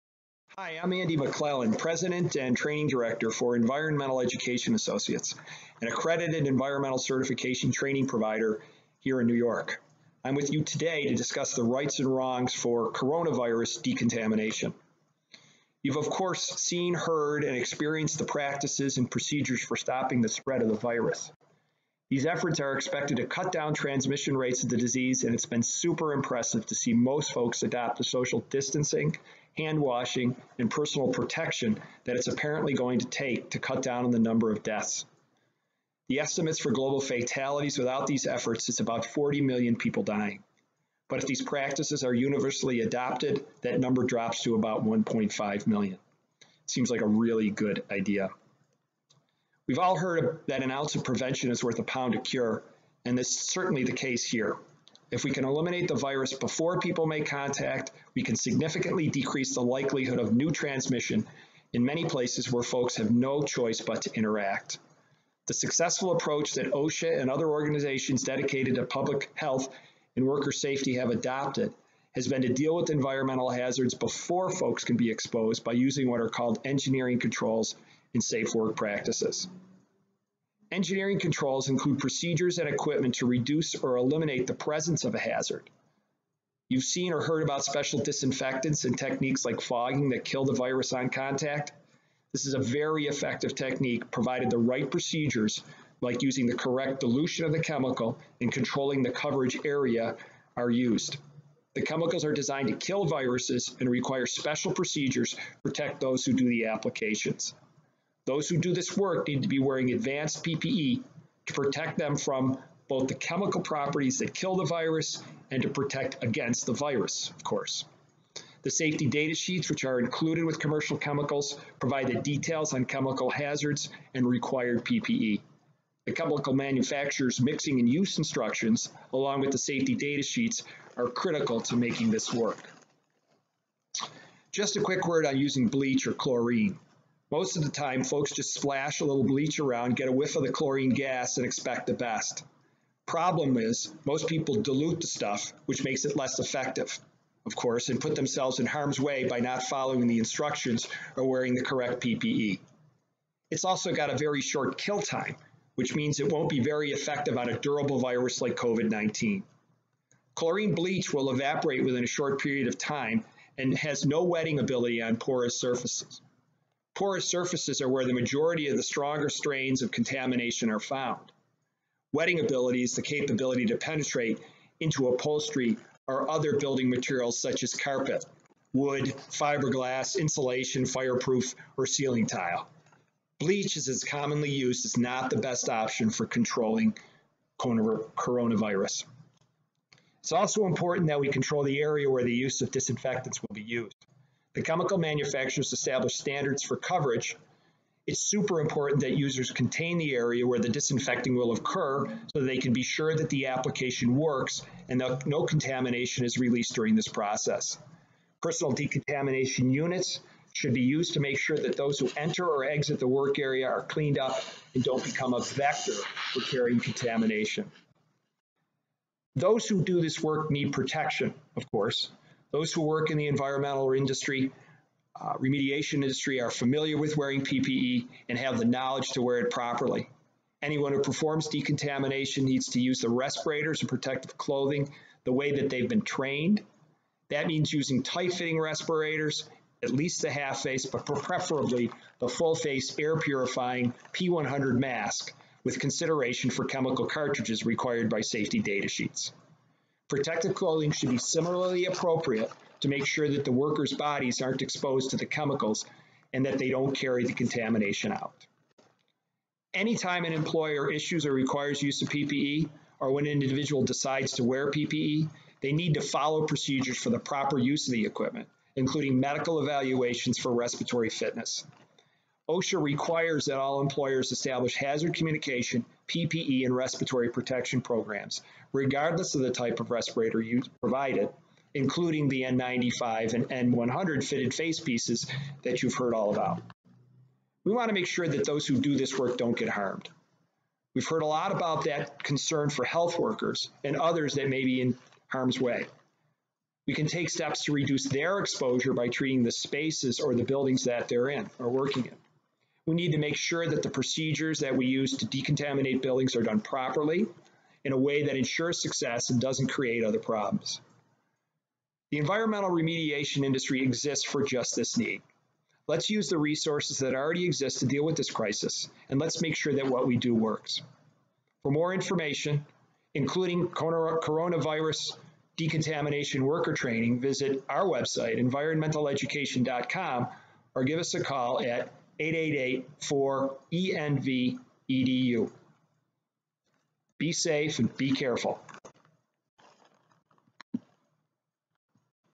COVID19: Decontamination Do's & Don'ts for Coronavirus
Learn the best practices for COVID 19 disinfection & decontamination from Andy McLellan, training director for Environmental Education Associates certification training programs.  Proper building decontamination is critical for making and keeping spaces safe during the Coronavirus pandemic   Proper work practices for decontamination include work area containment, engineering controls and special disinfectants for killing viruses.  Those who decontaminate buildings need advanced personal protective equipment (PPE) and must be familiar with correct use of disinfection chemicals.